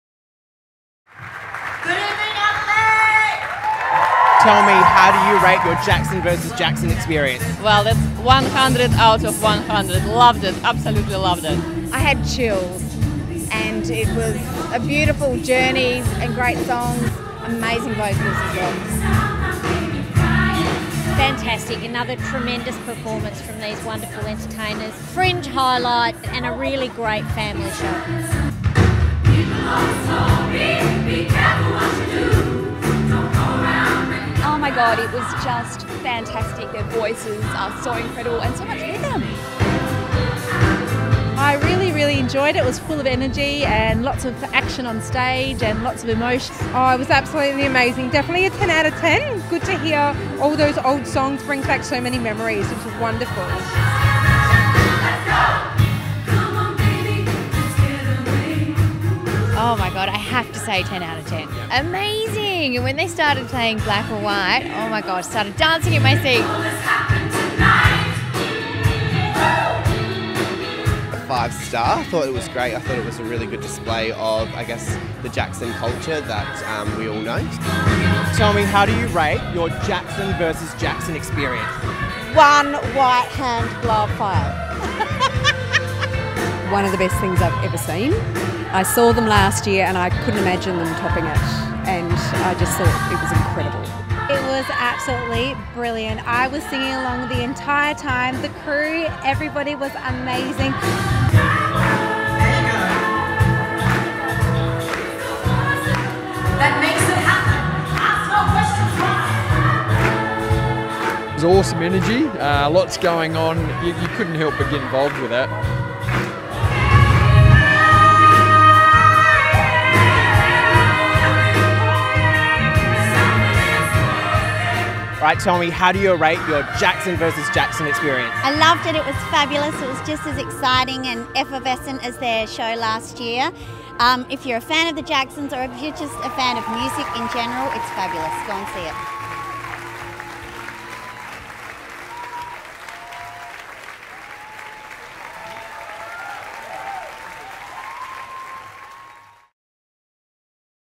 Jackson Vs Jackson | Opening Night 2018
The verdict is in... Filip With An F interviews guests from the opening night of Jackson Vs Jackson at Gluttony for Adelaide Fringe. Here's what they had to say!